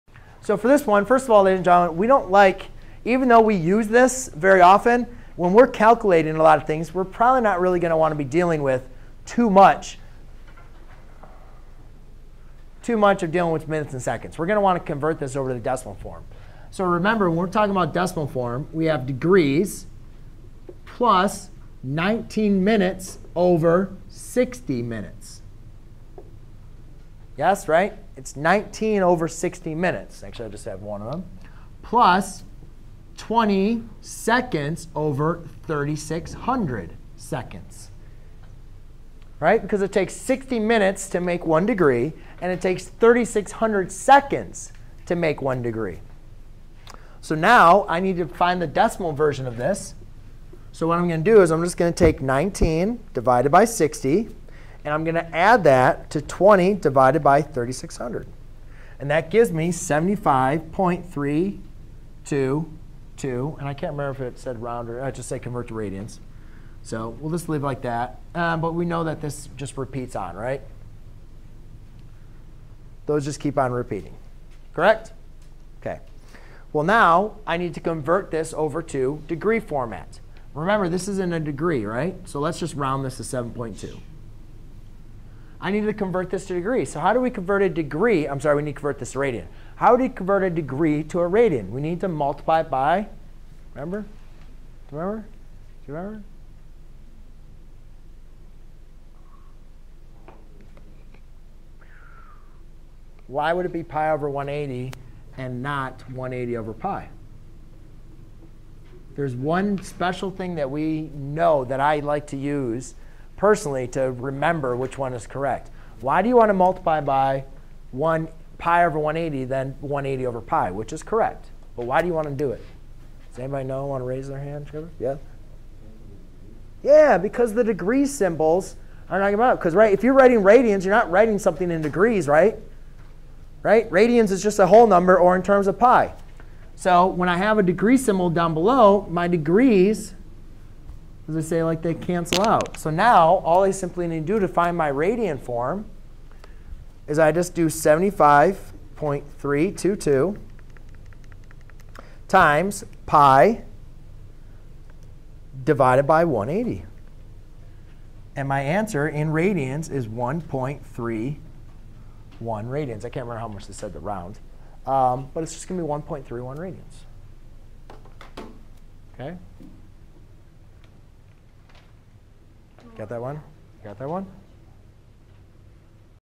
Convert an angle from degrees minutes and seconds to radian form
👉 Learn how to convert angles from DMS forms to decimal forms. DMS means Degree, Minutes, and Seconds. Minutes is represented by a single apostrophe (') while seconds is represented with double apostrophe ("). 60 seconds = 1 minute and 60 minutes = 1 degree.

To convert an angle from the DMS form to the decimal form, we add to the degree part of the DMS from the quotient of the minute part of the DMS form and 60 and the quotient of the seconds part of the DMS form and 3600. The resulting sum is the decimal form of the given DMS angle form.